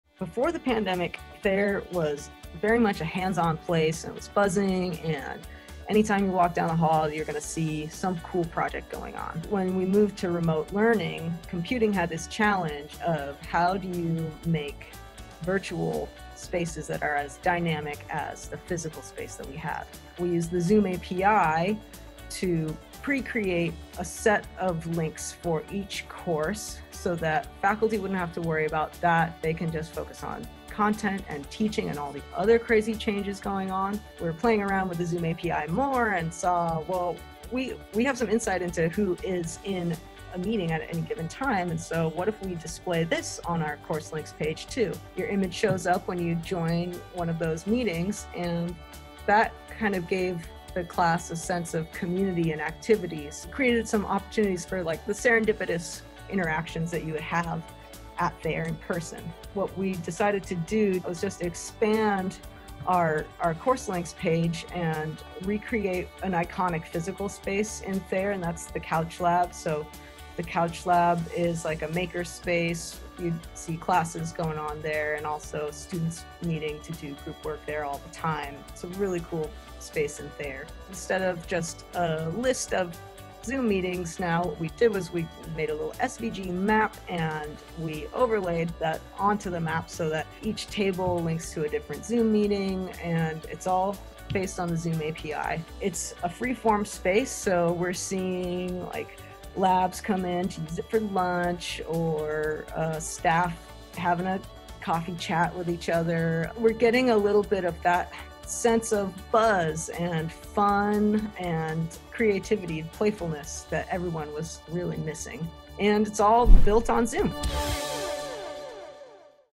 Dartmouth Engineering Virtual Project Lab
In an effort to make Thayer School just as dynamic digitally as it is in person, the Computing Services team, working with Thayer's graphic designer, built the Virtual Couch Project Design Lab. The informal digital meeting space helps maintain Thayer's "buzz" of hands-on learning and personal interaction.